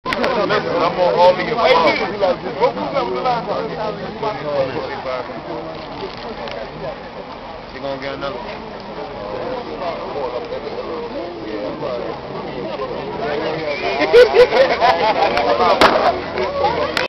RICK ROSS IN THE BRONX "CASTLE HILL PROJECTS"
ROSS DROPS BY CASTLE PROJECTS TO PROMOTE HIS NEW ALBUM AND FILM MUSIC VIDEO IN THE HOOD OF THE BRONX NOTOROUS HOUSING COMPLEX